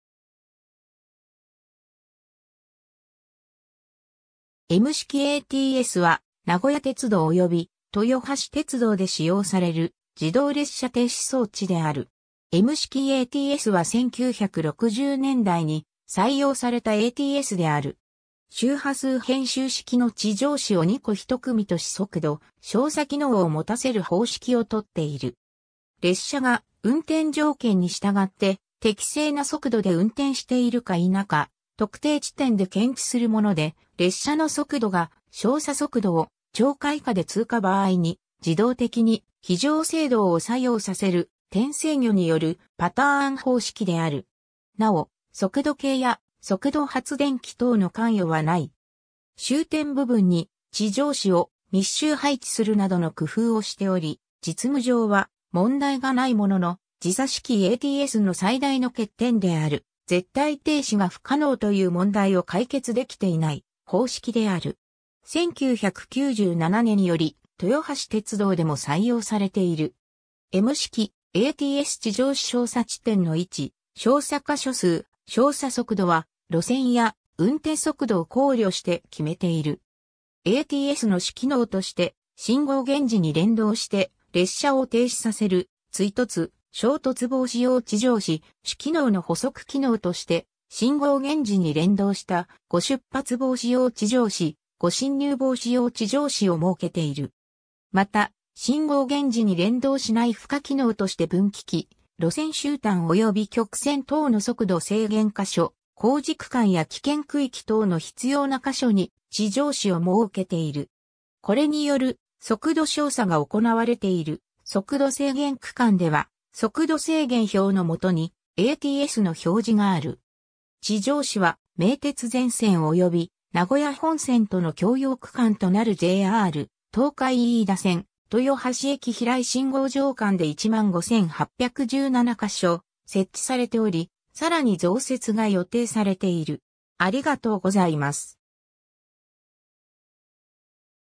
M式ATS
M式ATS（エムしきエーティーエス）は名古屋鉄道および豊橋鉄道で使用される自動列車停止装置 (ATS) である。
 M式ATSは1960年代に採用されたATSである。
周波数変周式の地上子を2個1組とし速度照査機能を持たせる方式をとっている。
 列車が運転条件に従って適正な速度で運転しているか否か特定地点で検知するもので、列車の速度が照査速度を超過（列車が2個の地上子間を一定の時間（0.5秒）以下で通過）場合に自動的に非常制動を作用させる、点制御によるパターン方式である。
なお、速度計や速度発電機等の関与は無い。
 終点部分に地上子を密集配置するなどの工夫をしており実務上は問題が無いものの、時差式ATSの最大の欠点である絶対停止が不可能という問題を解決出来ていない方式である。
 1997年より豊橋鉄道でも採用されている。
 M式ATS地上子 照査地点の位置、照査箇所数、照査速度は路線や運転速度を考慮して決めている。
 ATSの主機能として信号現示に連動して列車を停止させる追突・衝突防止用地上子、主機能の補足機能として信号現示に連動した誤出発防止用地上子、誤進入防止用地上子を設けている。
また、信号現示に連動しない付加機能として分岐器、路線終端および曲線等の速度制限箇所、工事区間や危険区域等の必要な箇所に地上子を設けている。
これによる速度照査が行われている速度制限区間（名古屋駅南方のトンネル入口など）では速度制限標の下にATSの表示がある。
 地上子は名鉄全線及び名古屋本線との共用区間となるJR東海飯田線・豊橋駅 - 平井信号場間（飯田線内はATS-PTと併設）で15,817箇所設置されており、さらに増設が予定されている。